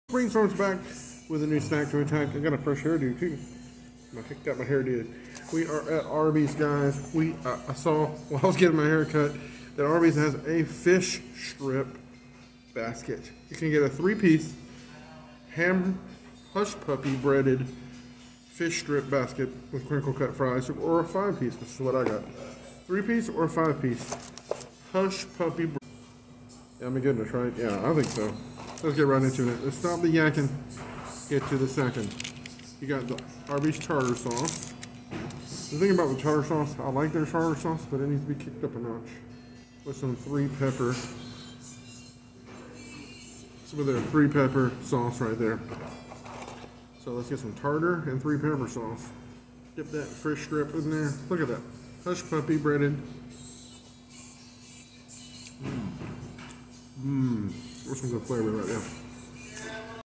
Hushpuppy Breaded FISH STRIPS at Arby's!!
Premium Pollock strips, battered in a crispy hushpuppy breading and deep fried. Limited time only at participating locations while supplies last. Try the 3-piece or 5-piece meal with either Arby's famous curly fries or crinkle cut fries and a drink!!